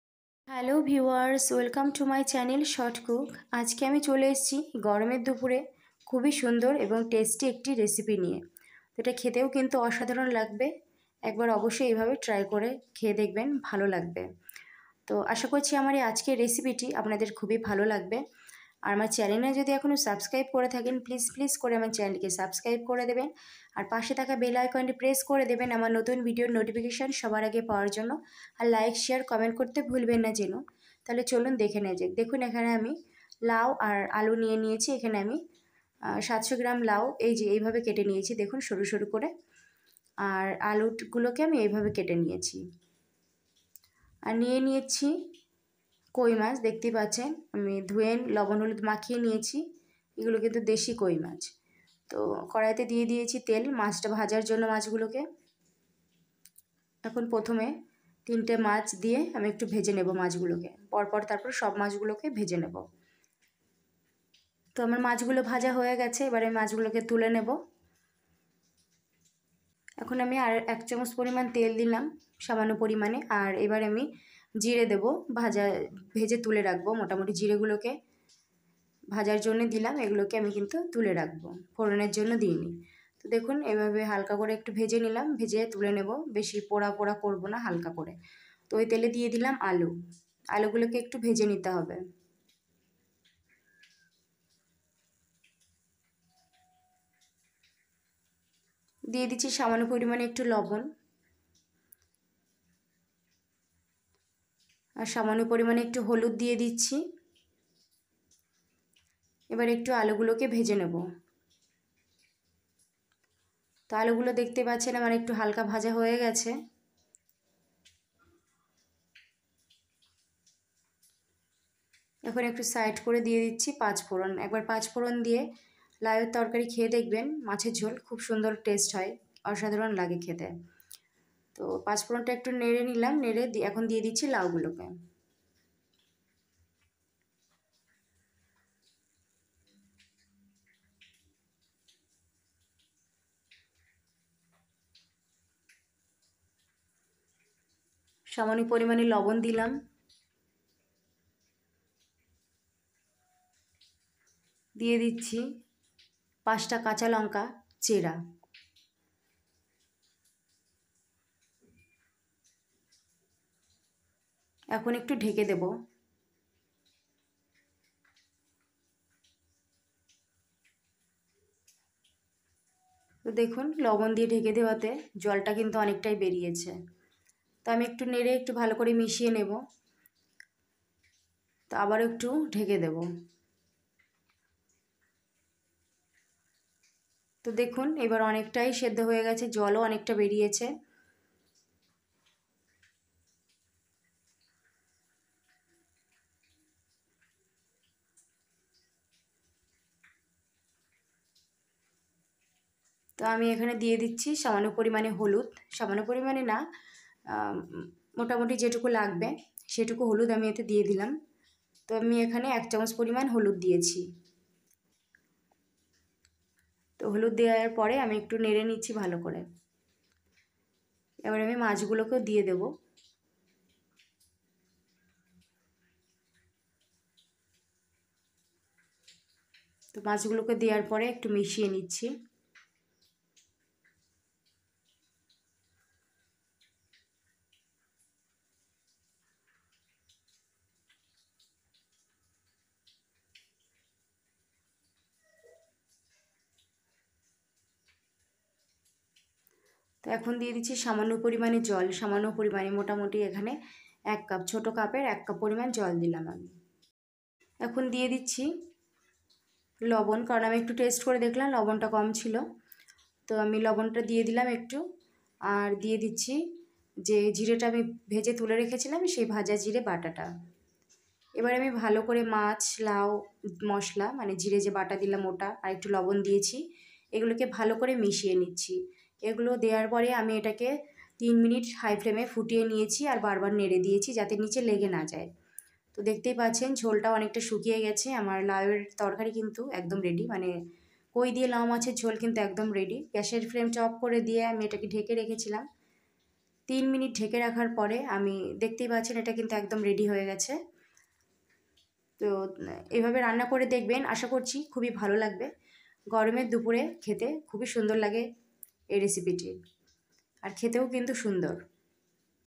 LAU KOI | লাউ দিয়ে কৈ মাছের ঝোল | কৈ মাছ রেসিপি
LAU KOI | KOI WITH LAU | লাউ দিয়ে কৈ মাছের ঝোল

 লাউ দিয়ে কৈ মাছের ঝোল ।এটি  গরমের দিনে খুবই  উপকারী একটি খাবার।এই ভাবে কই মাছ রান্না করলে ভালো লাগে। এটি খেতে কিন্তু খুব টেস্টি এবং শরীরের জন্য খুবই ভাল একটি খাবার।